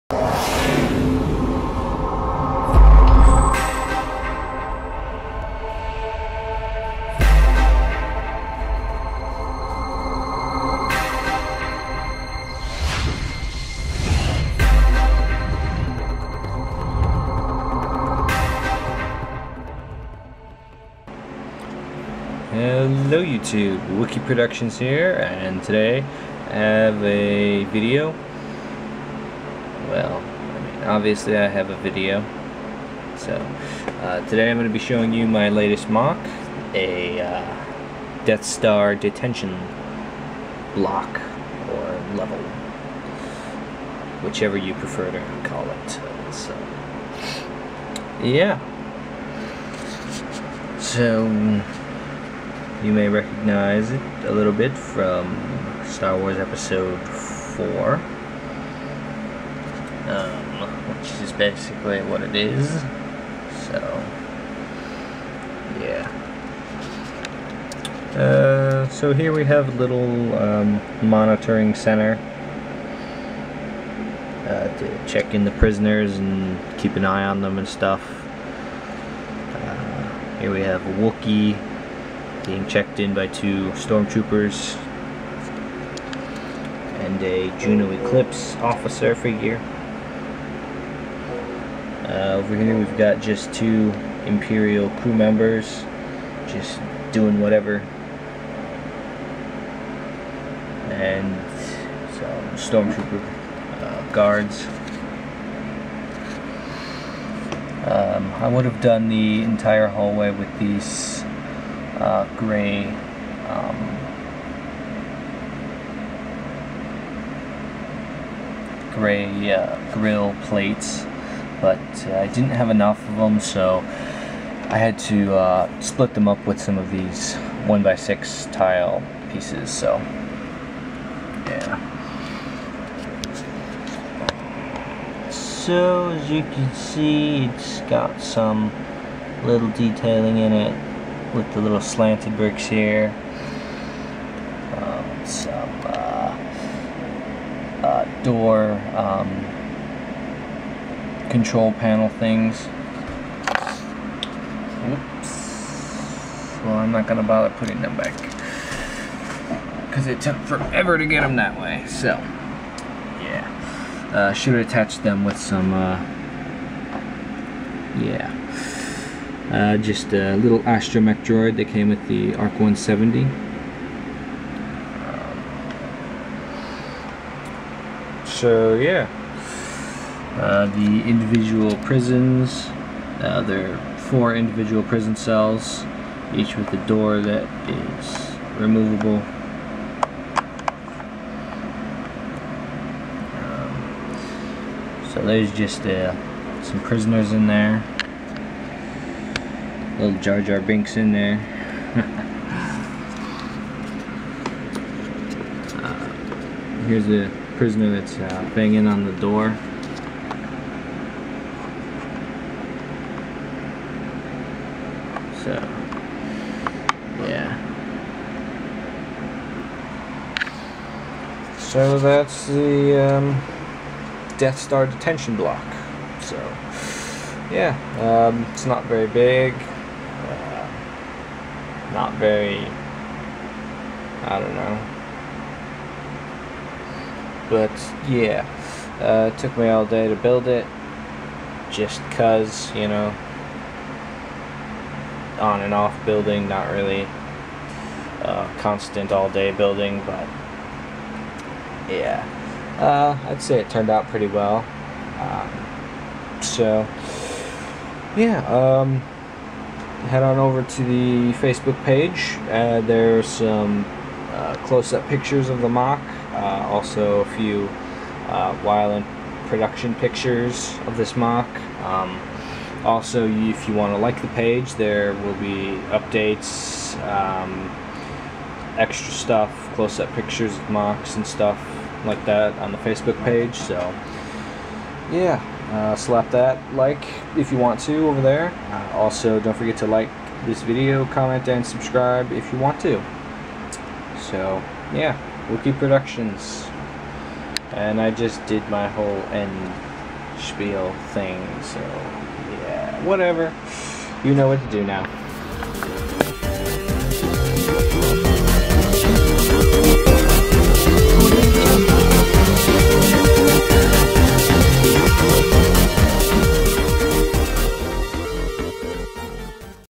Lego Star Wars Death Star Detention Level (moc)
built it in one day XD (I wonder how many times I said yeah, um and uh)